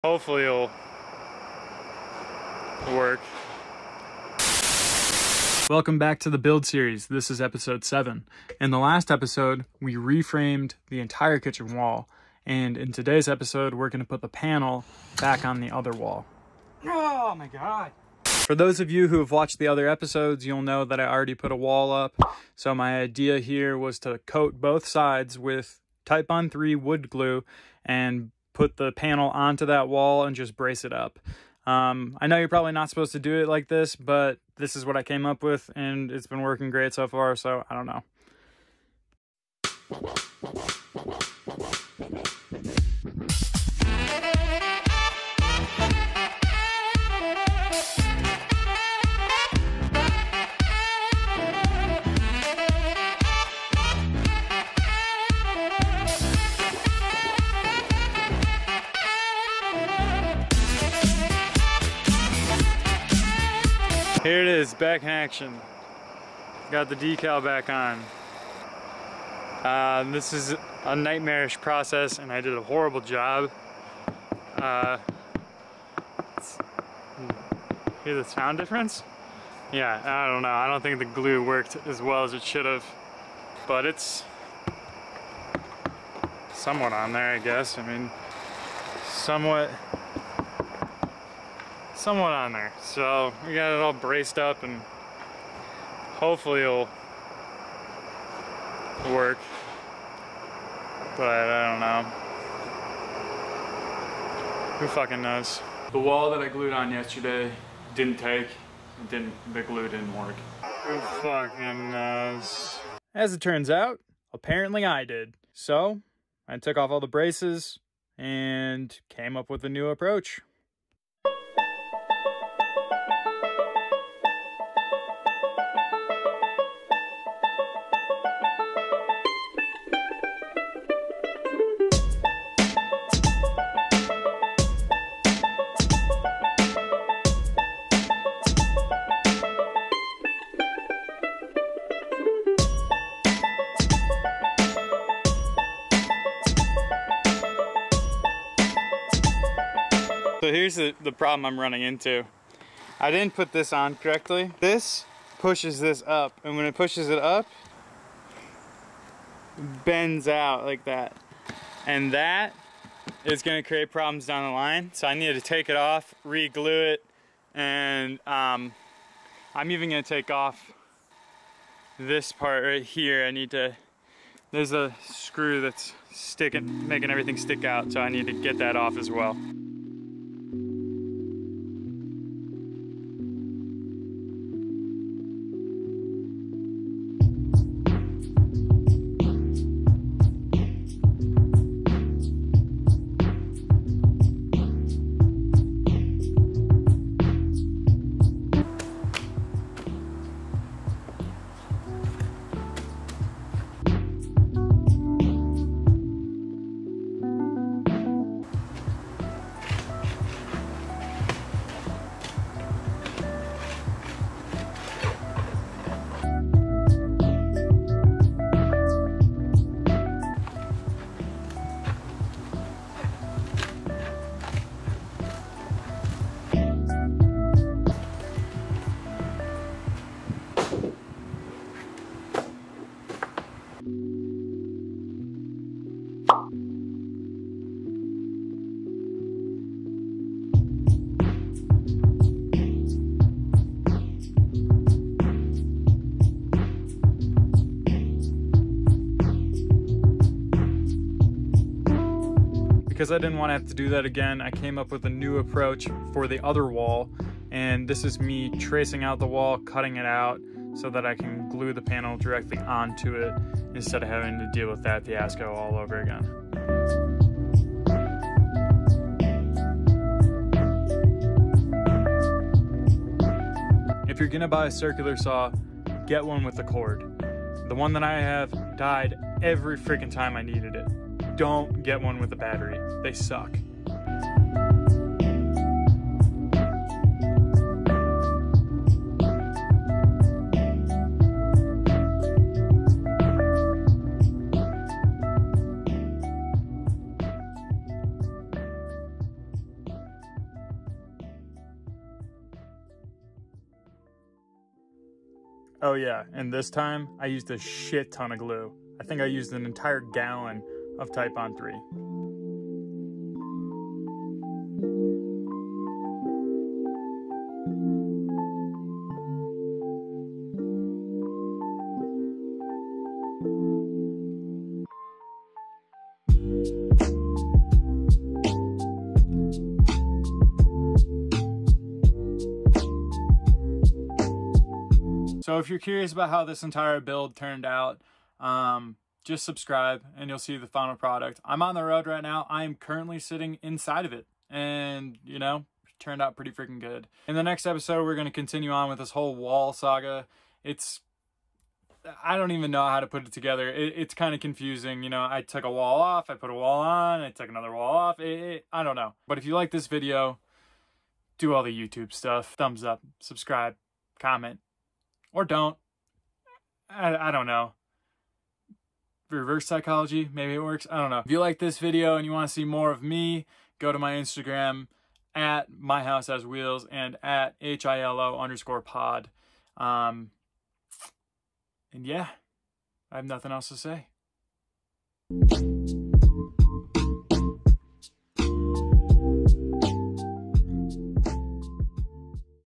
DIY RV TINYHOUSE CONVERSION-EP. 7: SORT OF PUTTING ON A WALL?
Putting on this wall did not go as planned.

Harris Heller